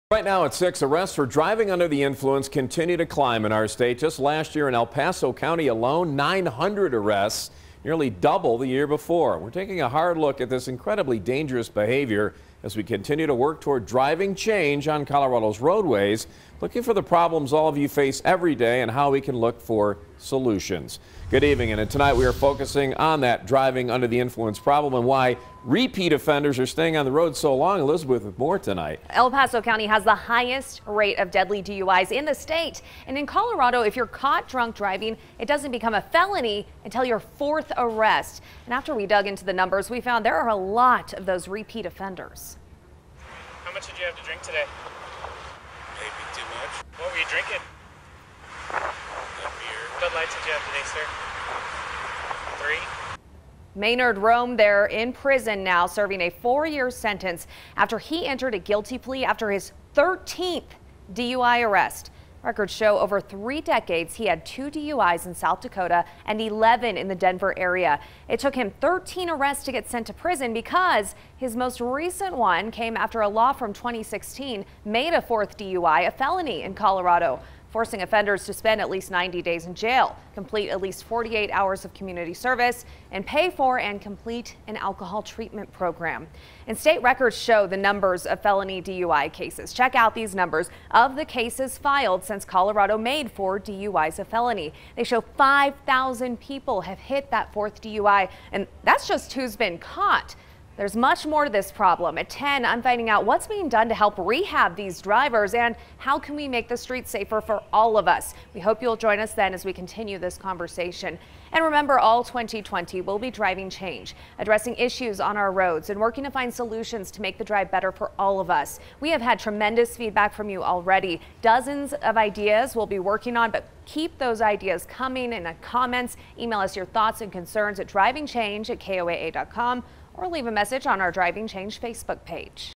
Driving Change: Repeat DUI Offenders
El Paso County often leads the state in not only DUI arrests but also deadly DUI crashes,  25 people were killed in 2019. News 5 has learned a lot of drunk or drugged drivers are repeat DUI offenders, but in Colorado, drunk driving is not a felony until your fourth arrest.